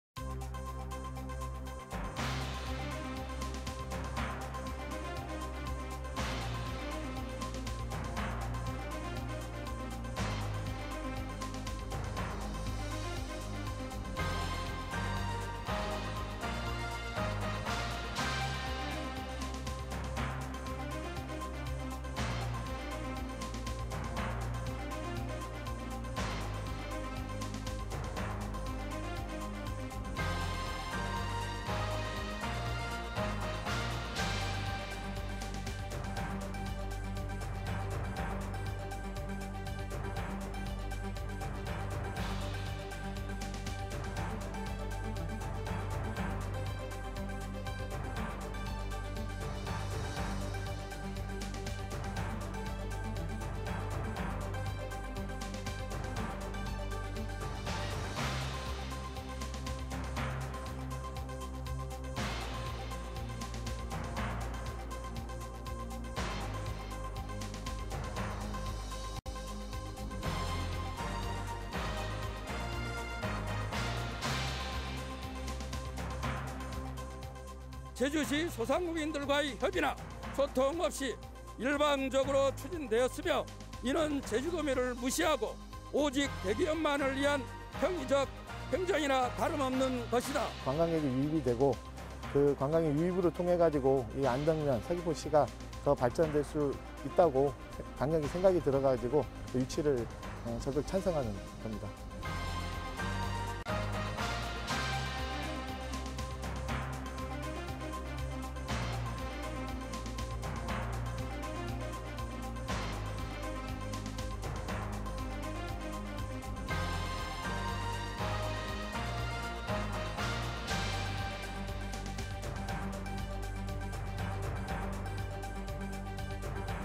미리보는 뉴스데스크 제주(2021.01.25/ 제주mbc)
1.
제주 코로나19 신규 확진 일주일 만에 0명

마라도 가는 여객선 승객 209명도 '전원 음성'

IM선교회 비인가 교육시설 '도내 활동은 없어'

2.
방역수칙 위반업소 38곳 적발

발한실, 매점 운영한 목욕업소 2곳에 '과태료'

불법 영업 '홀덤펍' 적발…제주도 특별점검

3.
신화월드 대규모 점포 허가 논란…찬반 갈등으로 번져

소상공인연합회 '반대' VS 지역 주민은 '찬성'

           ◀ＩＮＴ▶소상공인연합회
 "코로나로 힘든 시기에 허가 해주면 치명적"
           ◀ＩＮＴ▶지역주민
 "산남북 균형 발전 위해서 서귀포에도 쇼핑몰 필요하다"

자세한 소식은
오늘 밤 8시, 뉴스데스크 제주에서